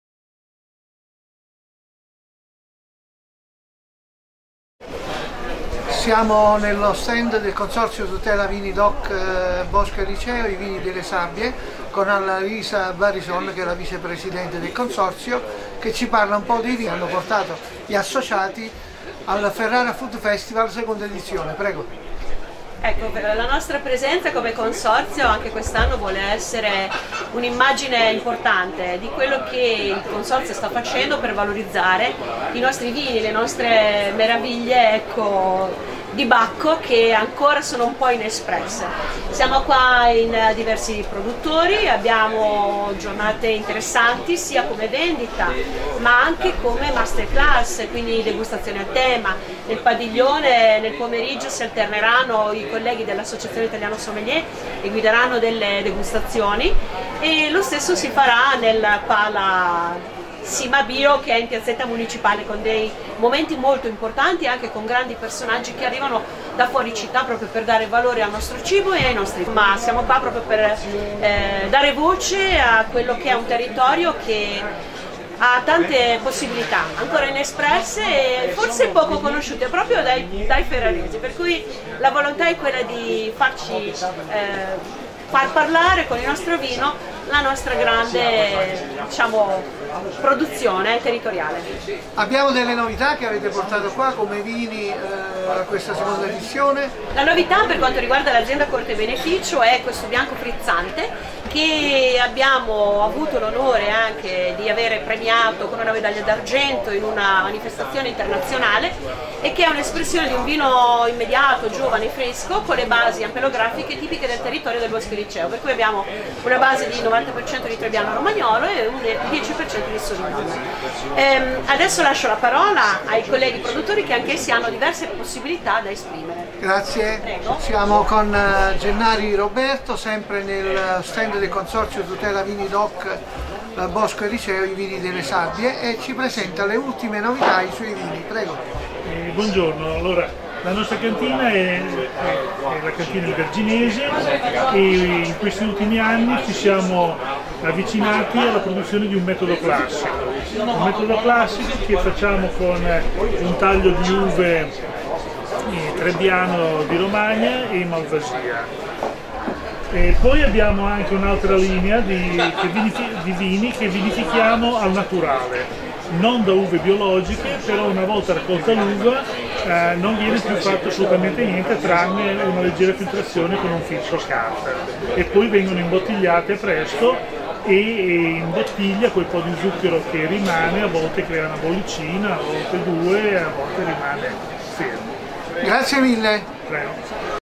Ferrara Food Festival 2022 - Annalisa Barison e Roberto Gennari
Intervista alla vicepreseidente del Consorzio Tutela Vini del Bosco Eliceo e Roberto Gennari, Azienda Agricola. Parte tecnica, Giovanni Tavassi.